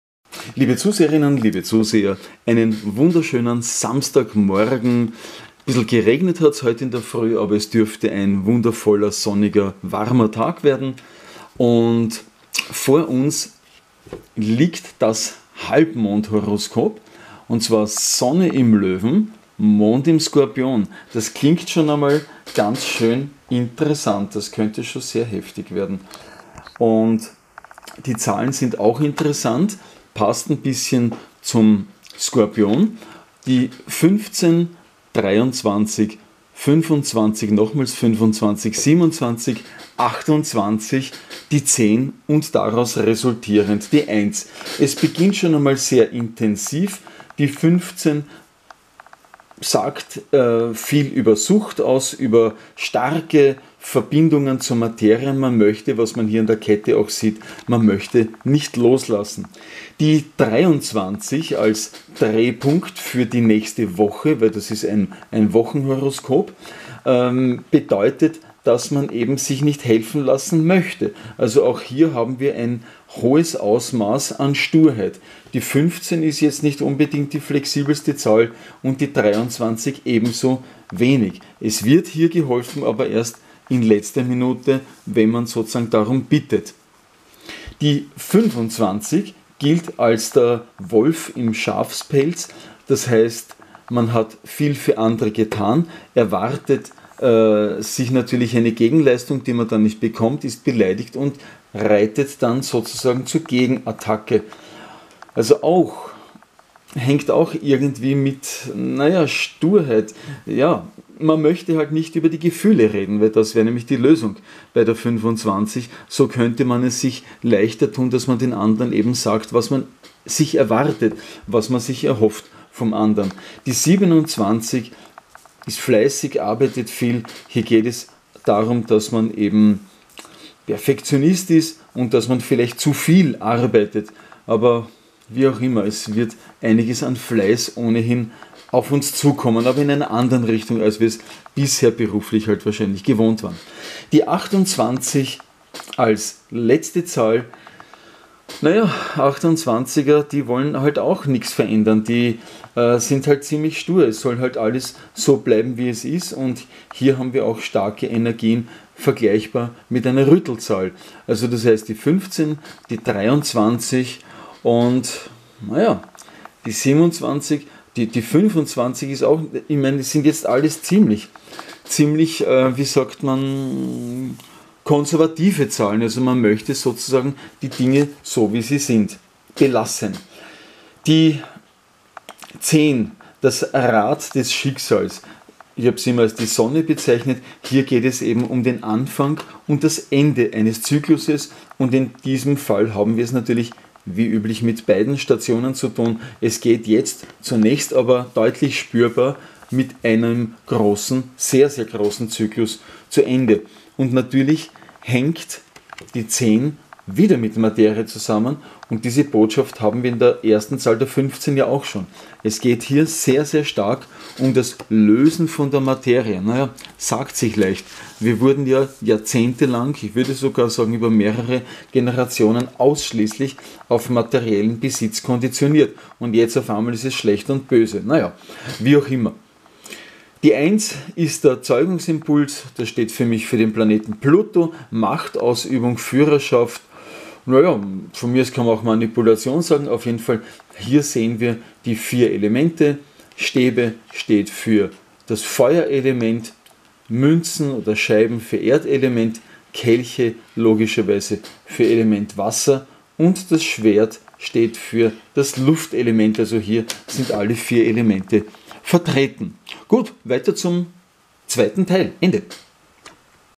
1/5 - HALBMOND im SKORPION - 15.08.2021 - BEHARRUNGSKRÄFTE erhöhen den LEIDENSDRUCK
Morgen ist ein "keltischer SATURN-Tag/Samstag" - das Horoskop gilt für 1 Woche.
Die XV steht für Intensität, für Sucht aber auch für Macht durch Magie. Hier fällt das Loslassen besonders schwer; wurden wir doch über Generationen auf das Thema "Besitz" konditioniert!
Die 23 will sich nicht gerne helfen lassen, und verharrt weiter stur in den alten Gewohnheiten. Erst in letzter Sekunde wird dann - oft fast schon zu spät - Hilfe angenommen. (Viele Selbständige haben natürlich ihren Stolz, und haben ja auch immer wieder Krisen gemeistert. Doch diese Nummer ist größer als alles, was wir bis dato gesehen haben...)
Die 25 weist auf jene, die viel für andere getan haben, aber dann enttäuscht wurden, und auf Rache sinnen. Die Lösung wäre einfach, doch sind leider viel zu viele total gehemmt, und haben eben Probleme mit dem Aussprechen ihrer Erwartungshaltungen. (Andererseits verständlich, denn wer lässt sich schon gerne in die Karten schauen?)
Die 27 steht für Fleiß und Perfektionismus. Unter dieser Zahl finden sich die "Arbeitstiere", welche ständig arbeiten müssen.
Die 28 steht für mich für maximale Sturheit. Hier will man keinerlei Veränderungen - alles soll so bleiben, wie es ist. Na das führt dann zu heftigen Problemen, denn wir befinden uns längst in einem dramatischen Wandlungsprozess!

Die X weist auf das Ende des alten und den Anfang eines neuen Zyklus hin.
Die I steht für den Zeugungsimpuls, für Macht und Neubeginn.